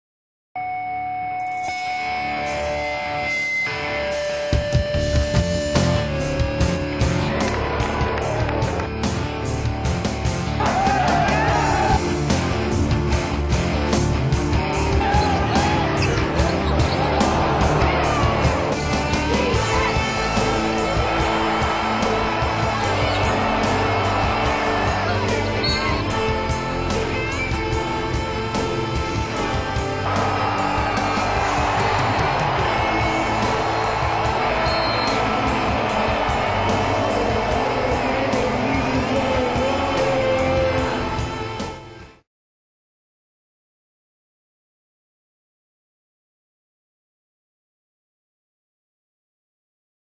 BASKETbrawl Of The Wild 2011-Montana Beats Montana State 75-61
Montage I did for the January 22, 2011 men's college basketball game between Montana State and Montana at Dahlberg Arena in Missoula.  In their first meeting of the season, the Griz dominated from the get-go, hammering their arch rival 75-61 in front of a sellout Dahlberg Arena crowd.